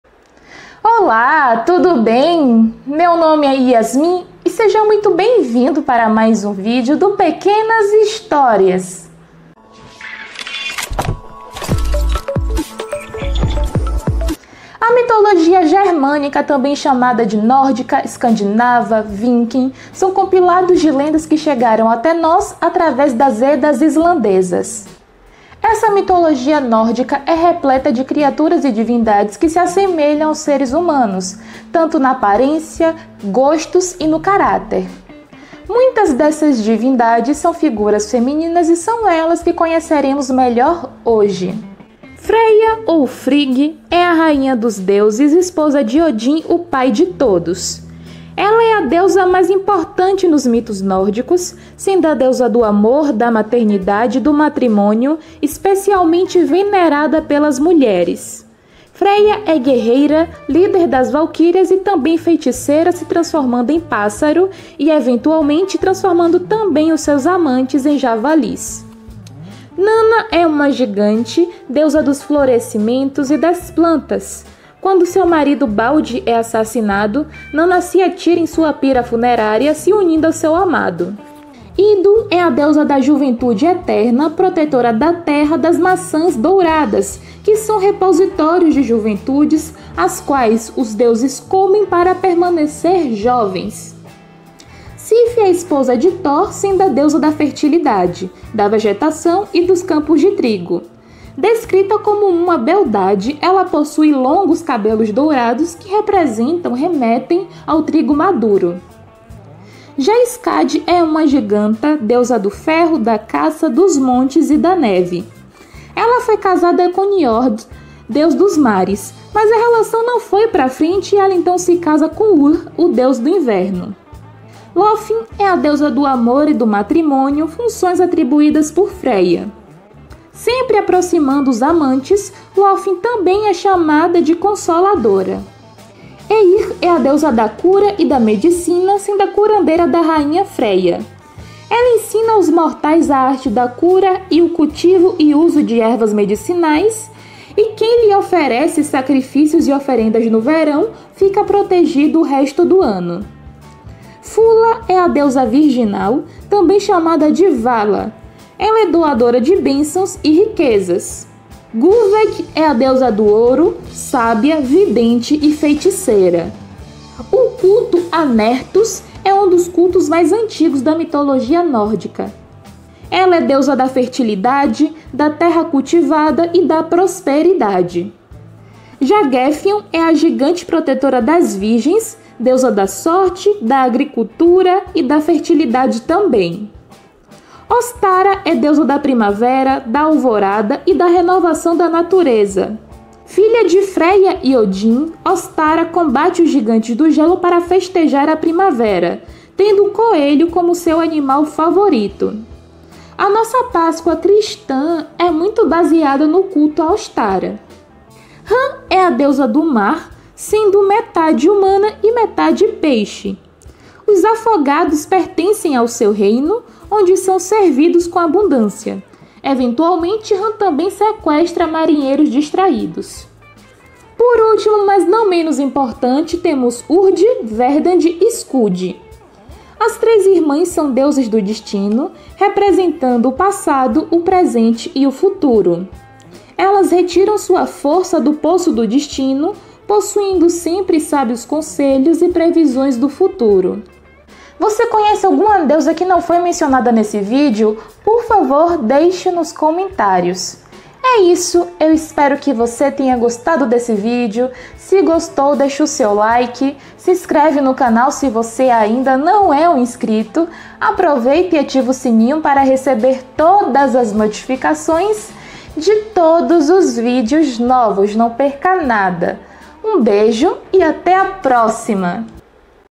MITOLOGIA 04/ Deusas nórdicas - Suas características e particularidades. - Yasmin Gomes.
A mitologia germânica, também chamada de nórdica, escandinava e viking, são lendas e histórias míticas que chegaram até nós, em maior parte, através das Edas islandesas do século XIII. Com gigantes e sereias, a mitologia nórdica é repleta de criaturas e divindades, cujas as escolhas e ações explicam o surgimento do mundo, as mudanças das estações, o ciclo da vida, desaparecimentos de pessoas, as atividades cotidianas, prevendo até o fim do mundo.
No vídeo de hoje, conheceremos um pouco mais das divindades femininas da mitologia germânica, em um apanhado das principais deusas nórdicas, então se você conhece alguma que não foi mencionada nesse vídeo, por favor, deixe nos comentários!

Insta pessoal: eu.yasgomes
Insta do canal: pequenashistorias.insta

- Deusas germânicas.
- Deusas nórdicas.
- Deusas vikings.
- Deusas pagãs.
- Mitologia nórdica.
- Mitologia germânica.
- Mitologia escandinava.
- Mitologia viking.